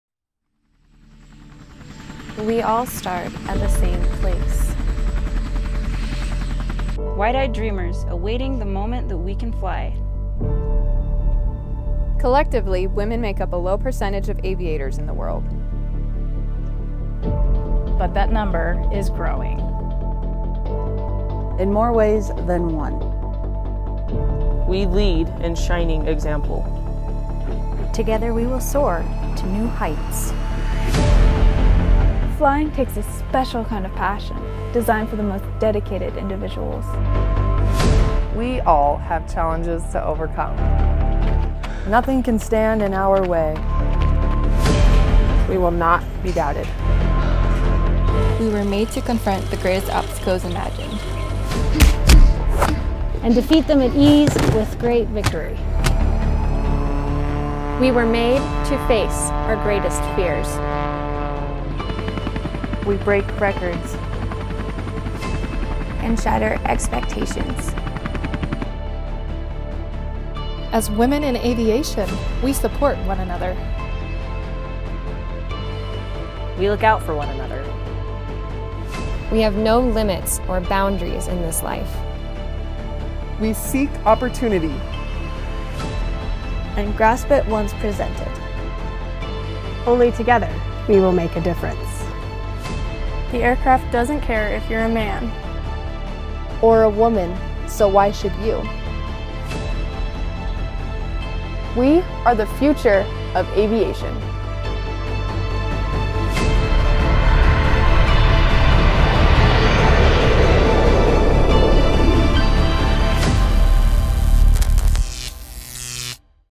SUU's Women in Aviation
In this video we highlight the women in all of SUU's Aviation Programs. Women make up a low percentage of the aviation community. The Women in Aviation conference is this week and we're putting out this video support the organization and all female aviators.

Apply to the program (usually takes less than 10 minutes)

All statements and claims made within this video are accurate as of the date of publication.